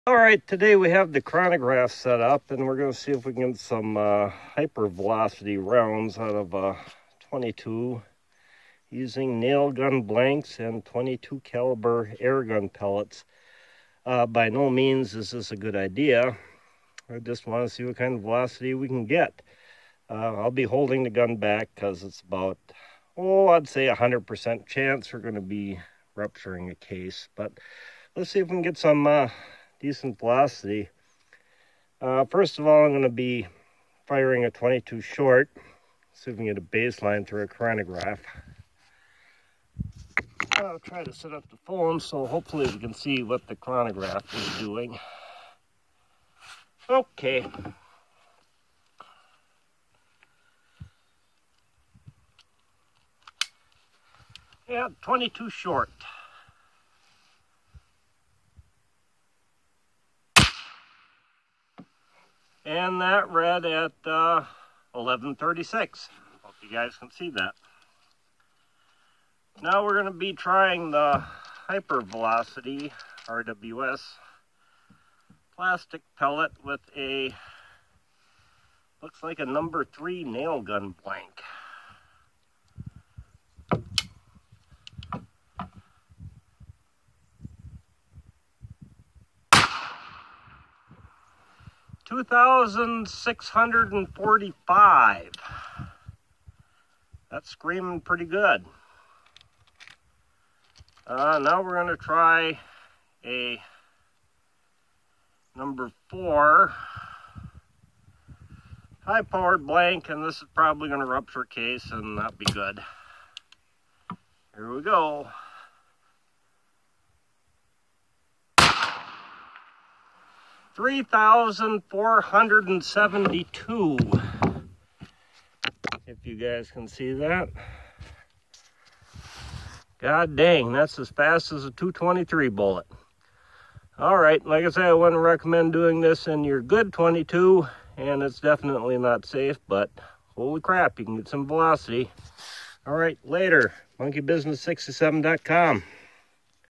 Nail gun blank and pellet from 22lr
Velocity test of nail gun blank and pellet from 22